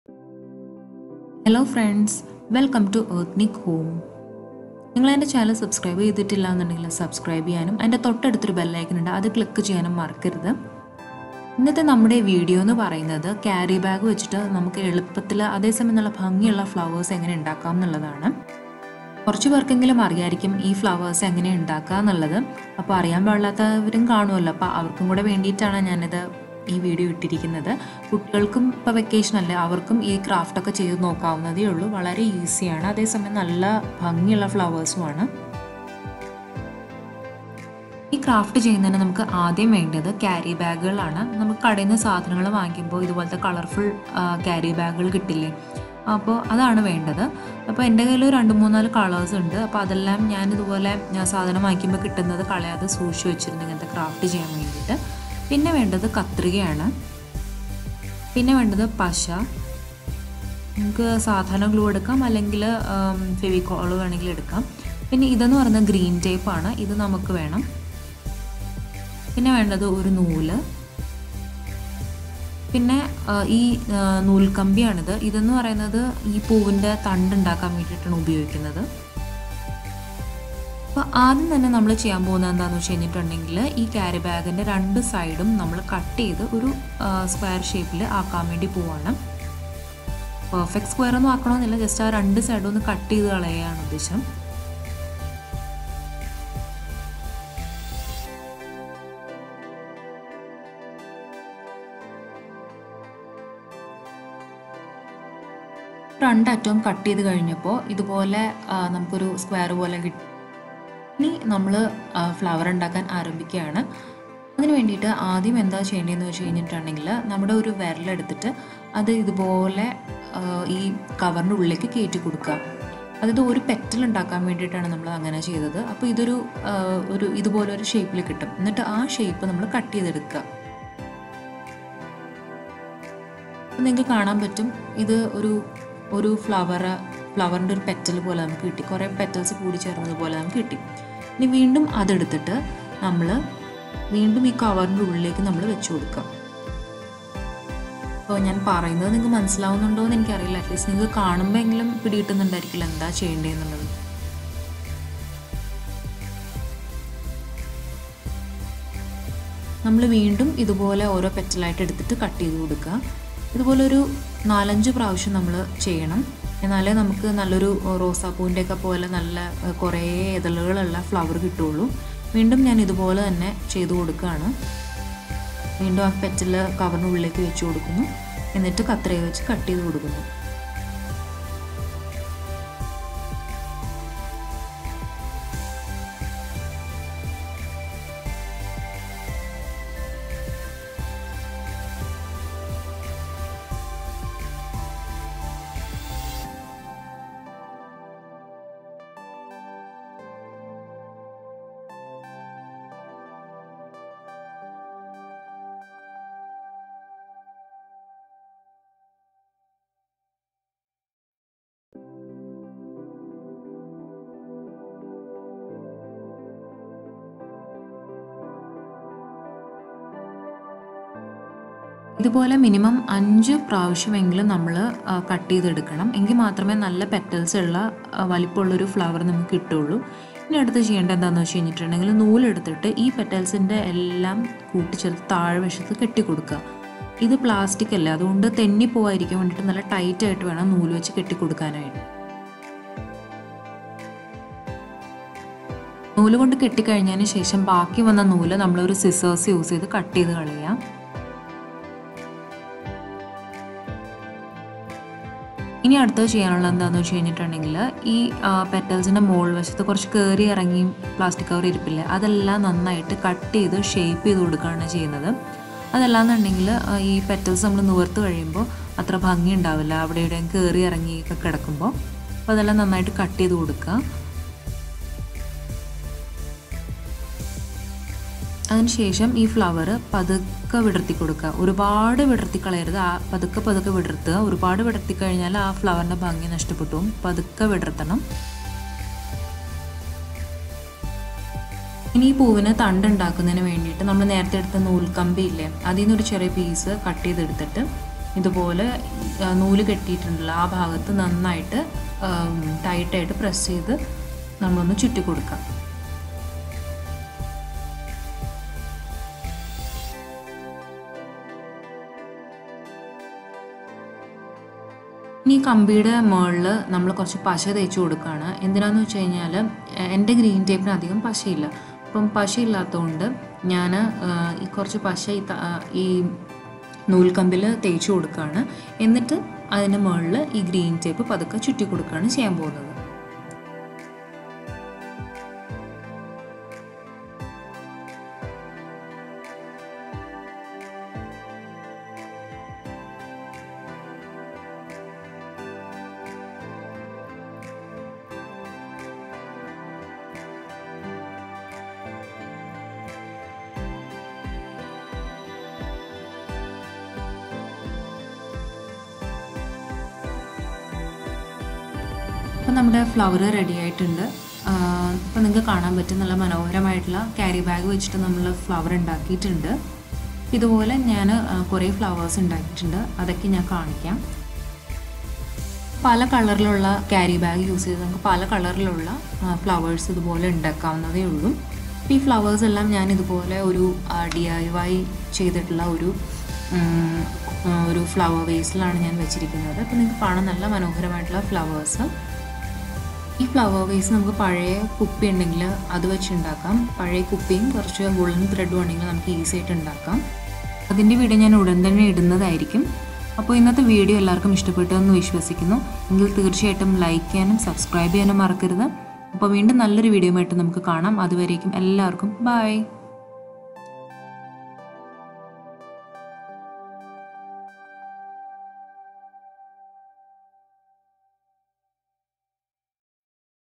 Easy DIY flower using Carry bag
This video demonstrates how to make flower using carry bag.